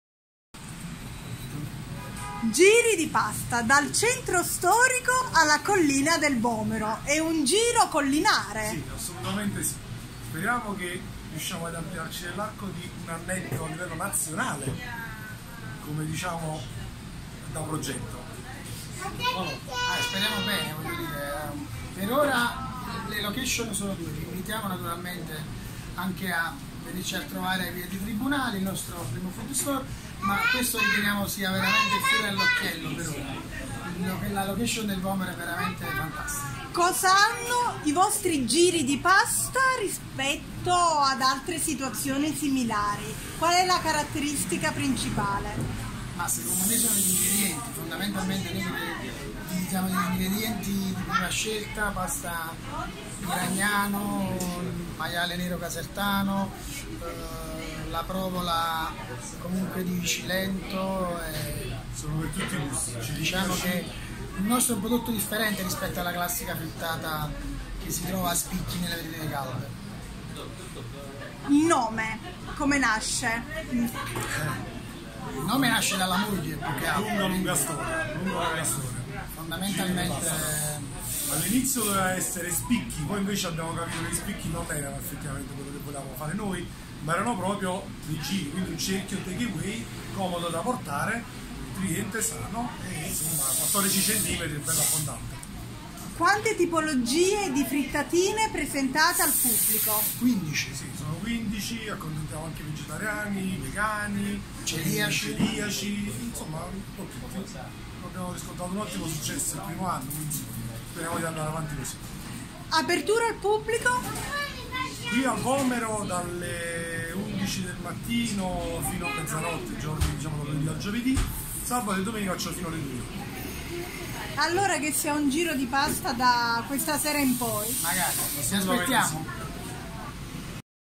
GIRIDIPASTA apre al Vomero
Napoli: Ha inaugurato a Via Bernini n.12, al Vomero, nei pressi di Piazza Vanvitelli, il secondo foodstore di Giridipasta che ha da via dei Tribunali replica in collina l'esperienza con il formta 'giridipasta'. Nell'intervista Dario Savarese e Eduardo Manca raccontano il concept store.